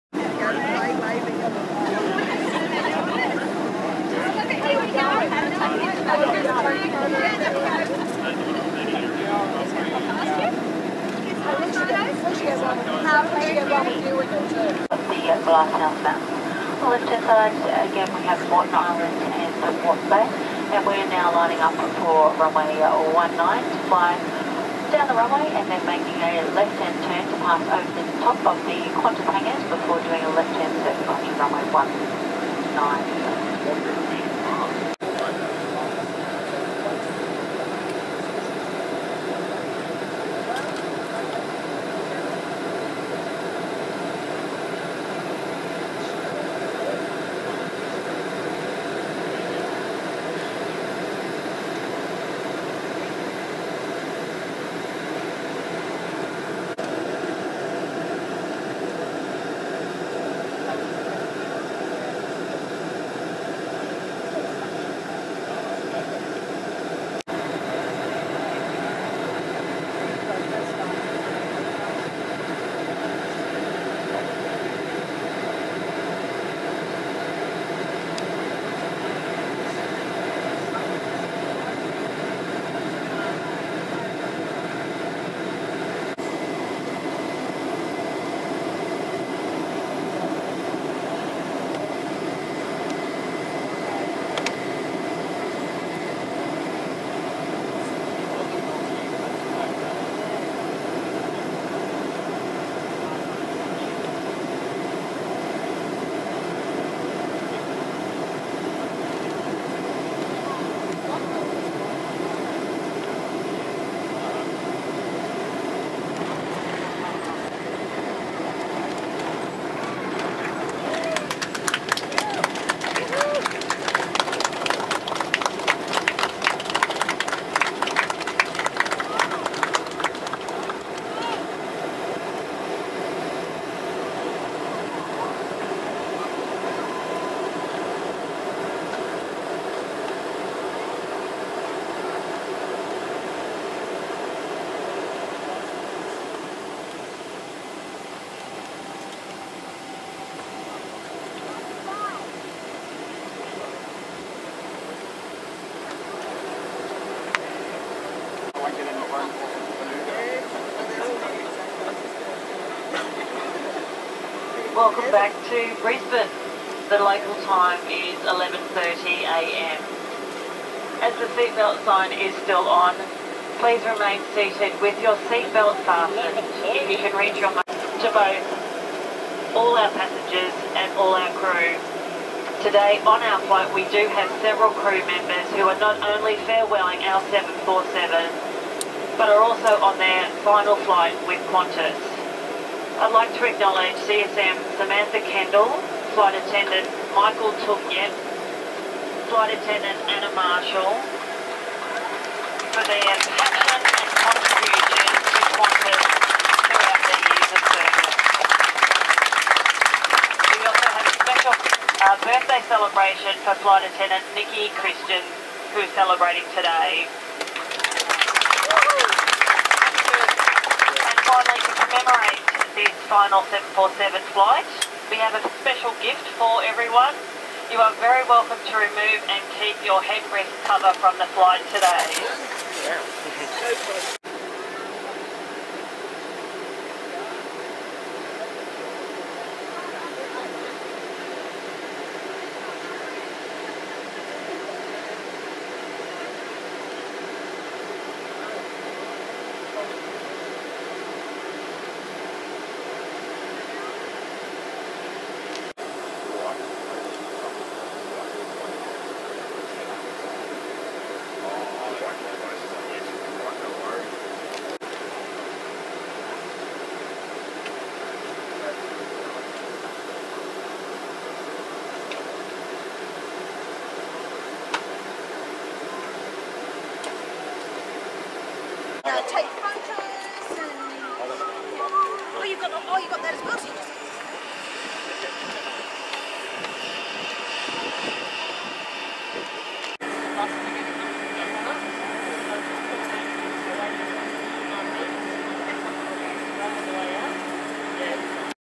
Qantas Boeing 747 LAST EVER Landing At Brisbane Airport | VH-OEJ WUNALA
Hello everyone. I have decided to upload a seperate video of the landing and taxi of the last ever Qantas 747 flight to land at Brisbane Airport, on July 15th 2020. The main video of the whole flight and experience can be found and if you haven't watched that yet, I reccomend watching it and giving the Qantas Queen some love! Long live the Qantas Boeing 747!